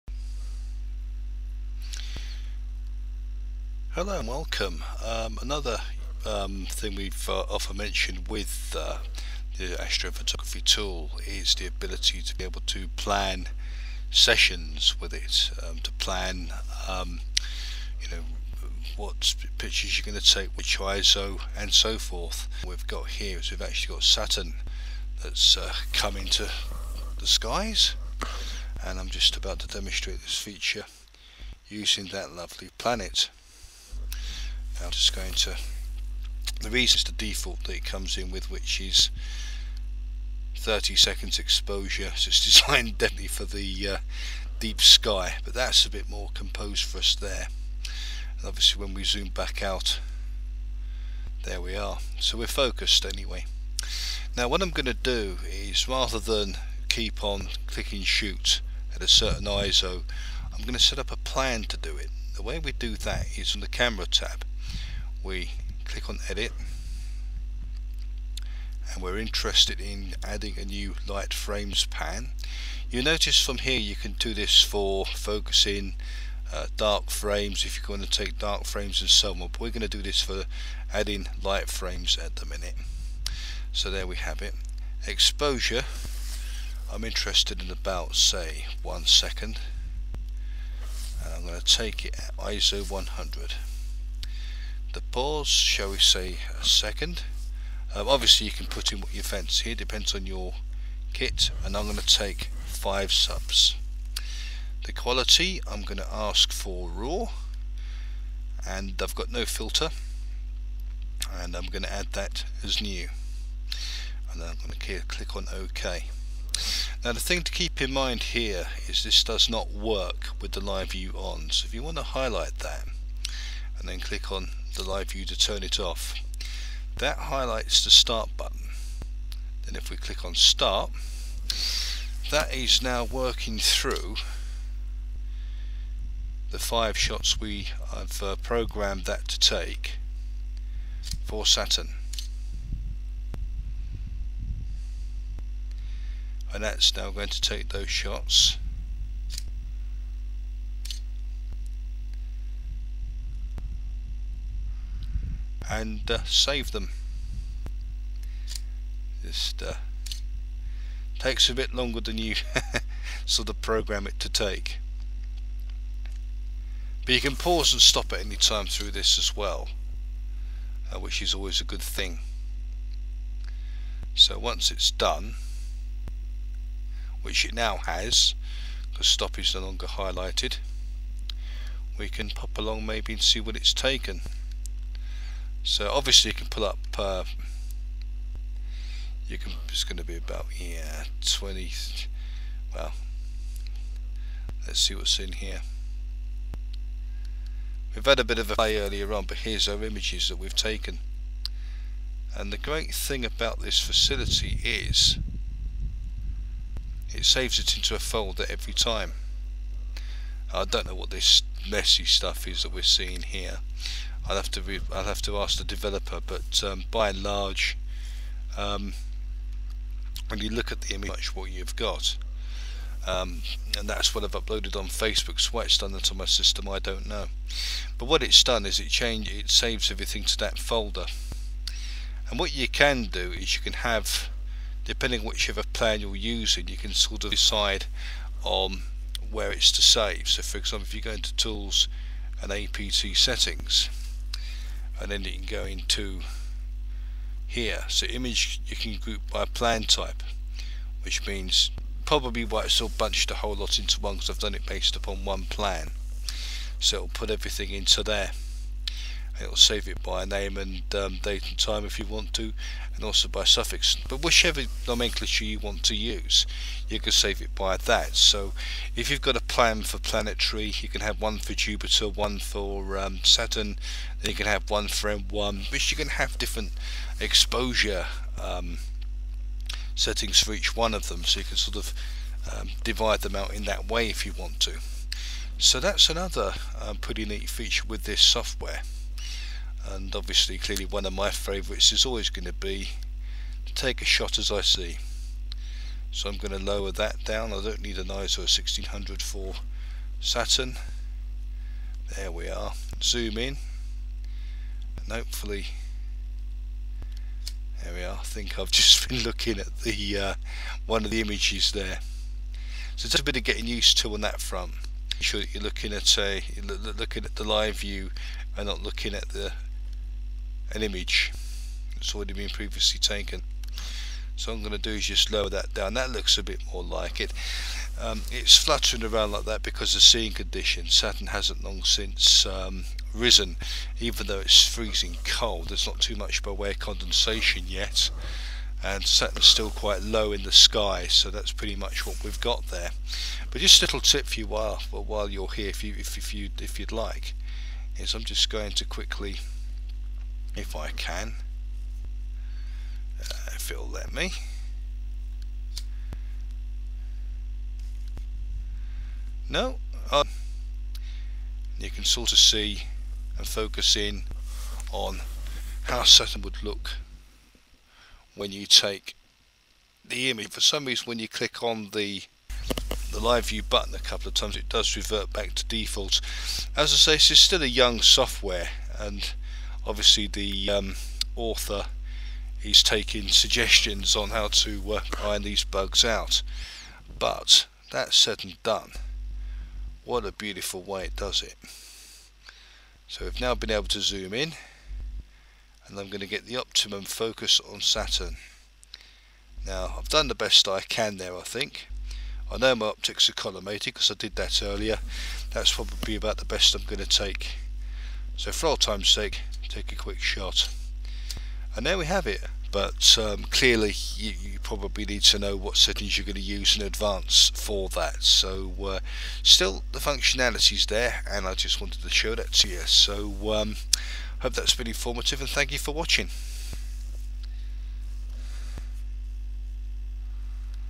APT - Video 3 creating observing plans
Next in the series of video tutorials showing how to create observing plans in Astro Photography tool. material created by Alastair of